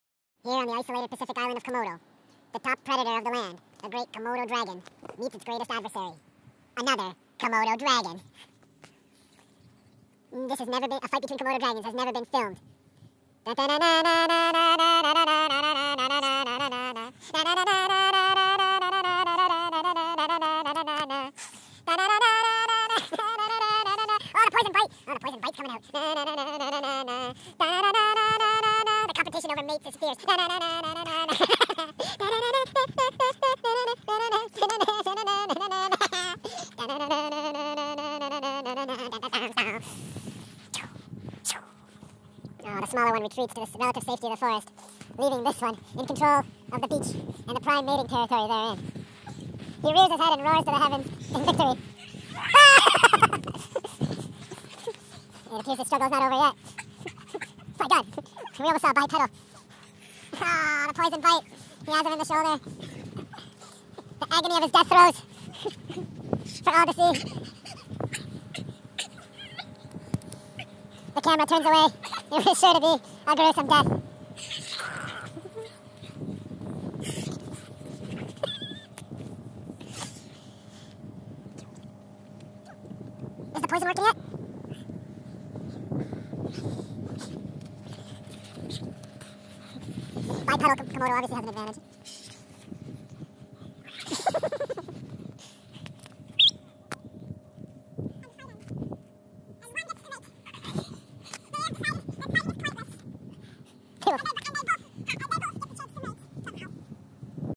faster by 2x komodo dragon fight
Rare vicious Komodo dragon fight caught in the wild to the death.   Young children do not watch.  Extreme.  Wild.  Wow.